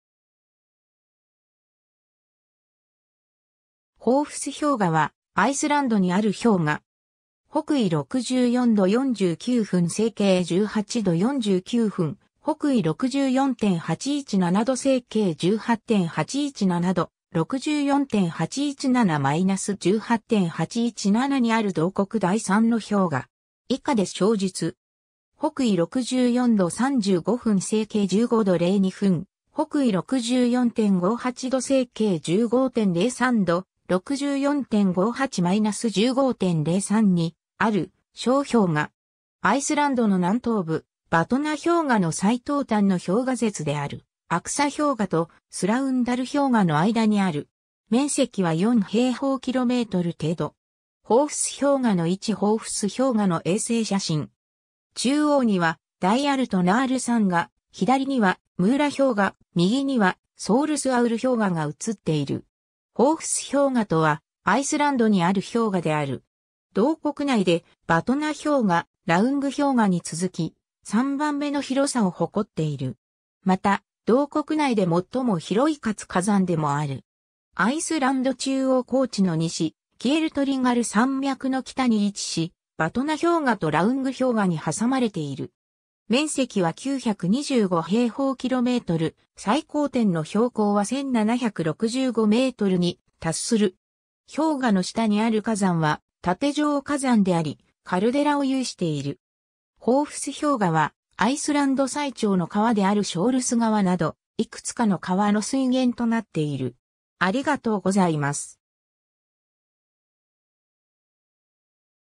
ホーフス氷河
ホーフス氷河（アイスランド語: Hofsjökull, 「聖堂の氷河」の意）は、アイスランドにある氷河。
 北緯64度49分 西経18度49分 / 北緯64.817度 西経18.817度 / 64.817; -18.817 にある同国第3の氷河。
以下で詳述。
 北緯64度35分 西経15度02分 / 北緯64.58度 西経15.03度 / 64.58; -15.03 にある小氷河。
アイスランドの南東部、ヴァトナ氷河の最東端の氷河舌であるアクサ氷河（アイスランド語: Axajökull）と、スラウンダル氷河（アイスランド語: Þrándarjökull）の間にある。
面積は 4 km2 程度。
 ホーフス氷河の位置 ホーフス氷河の衛星写真。
 中央には大アルトナール山（アイスランド語: Arnarfell hiðmikla, アルトナールフェットル・ヒズ・ミクラ、「鷲の大きな山」の意）が、左にはムーラ氷河（アイスランド語: Múlajökull）、右にはソウルスアウル氷河（アイスランド語: Þjórsárjökull）が写っている。
 ホーフス氷河（アイスランド語: Hofsjökull, 「聖堂の氷河」の意）とは、アイスランドにある氷河である。
同国内で、ヴァトナ氷河、ラウング氷河に続き、3番目の広さを誇っている。
また、同国内で最も広い活火山でもある。
 アイスランド中央高地の西、キェルトリンガル山脈（アイスランド語: Kerlingarfjöll）の北に位置し、ヴァトナ氷河とラウング氷河に挟まれている。
面積は 925 km2、最高点の標高は 1,765 m に達する。
氷河の下にある火山は楯状火山であり、カルデラを有している。
 ホーフス氷河は、アイスランド最長の川であるショウルス川（アイスランド語: Þjórsá）など、いくつかの川の水源となっている。